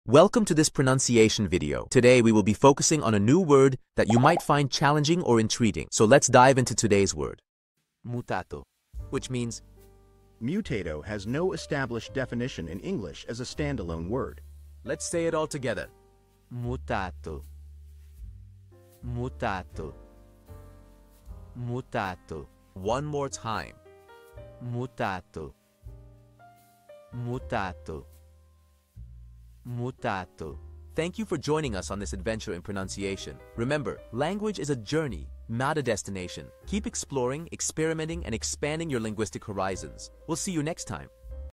How to pronounce Mutato
Master the Pronunciation of 'Mutato Which means changed' - which means : Changed has no established definition in English as a standalone word. 📝 with @PizzaPronunciation 🎓🗣️ - [Your Guide to Italian]

Learn to pronounce 'Mutato' perfectly in Italian with our easy and engaging tutorial by @PizzaPronunciation! 📖📽️ In this comprehensive video, we provide you with native speaker examples 🗣️👂 and phonetic breakdowns 🎵 to ensure you get the pronunciation just right.

How to say Changed in Italian